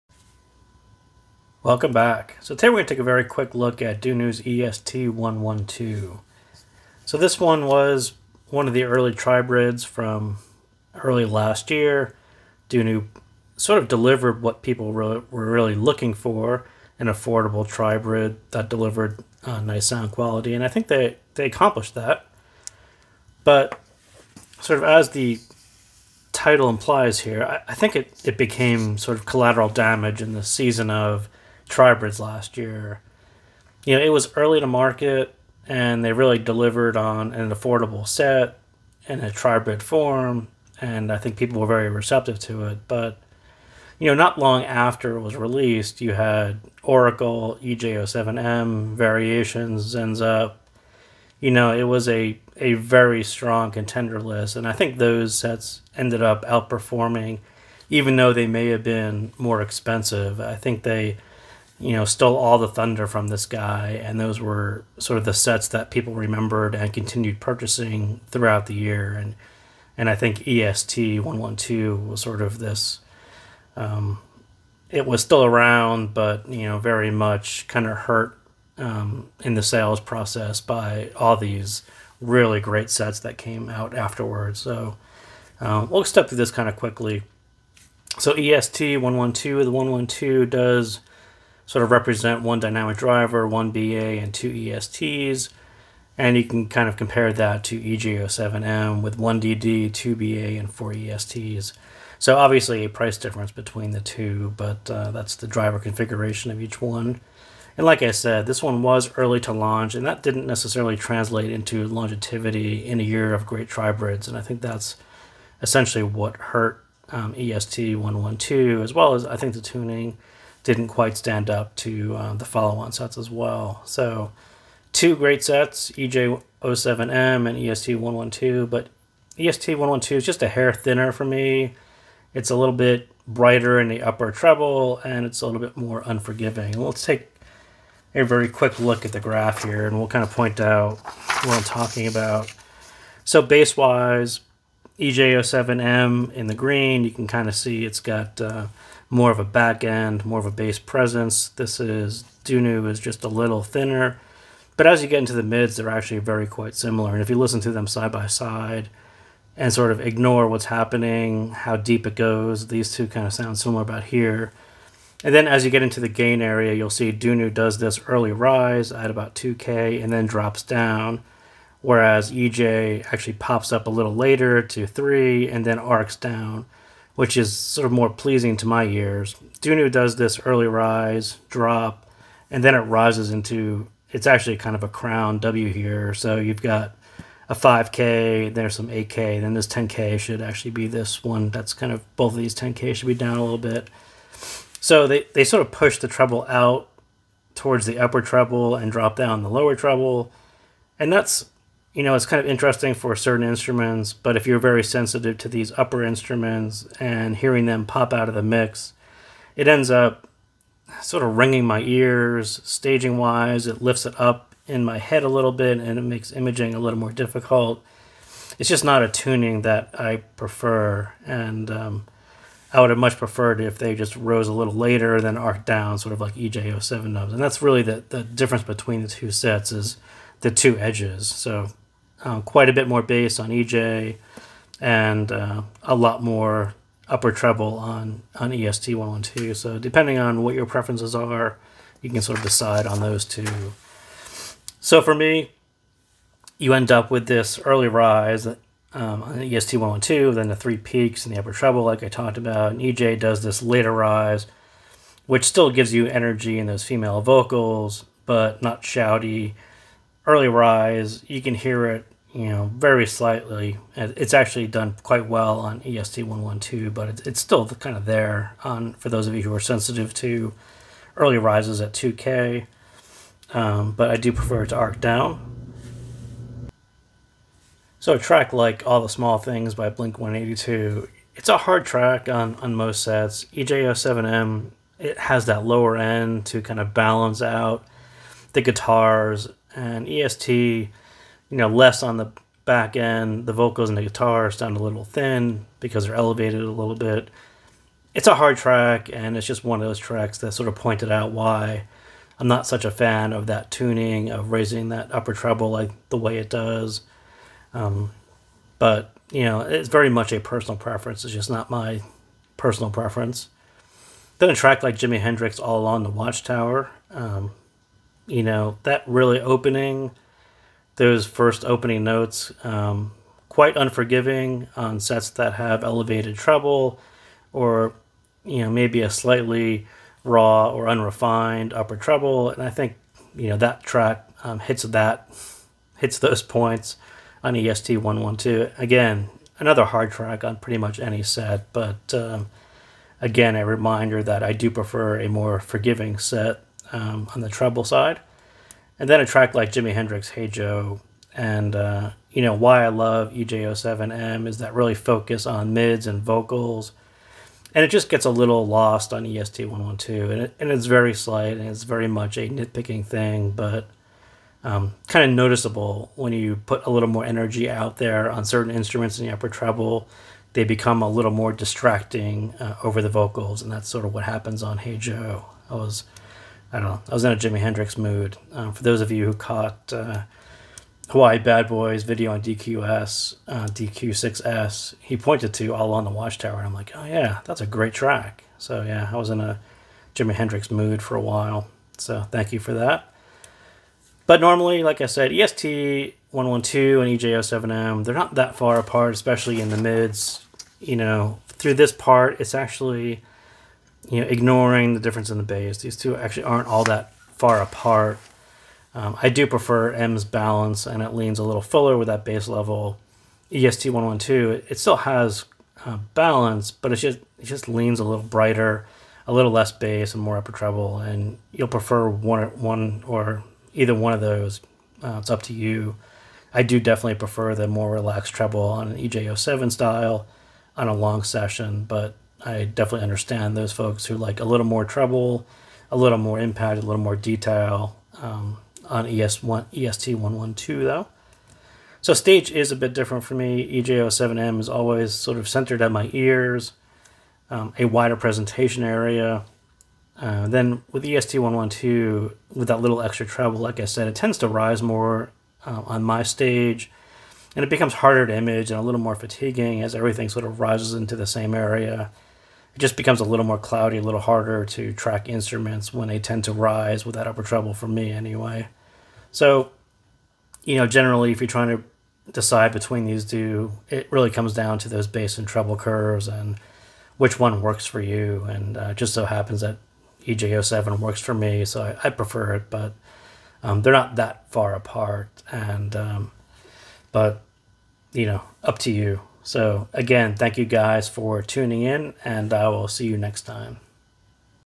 Quick Thoughts On...Dunu EST 112
About 11 months ago, DUNU released the EST 112 and it was very well received.  Shortly afterwards, more tribrids were released and I do think EST 112 was lost in the group of amazing sets.  It is certainly not bad, just perhaps not strong enough to compete solely on price.

Timecodes:
00:00 Intro and Close Up
00:25 Collateral Damage
01:28 Drivers
02:05 EST112 vs EJ07M
02:22 Graph
04:10 Tracks
06:34 Balance
07:23 Stage

The DUNU EST 112 was a loaner so thank you to the generous man who sent this over to me.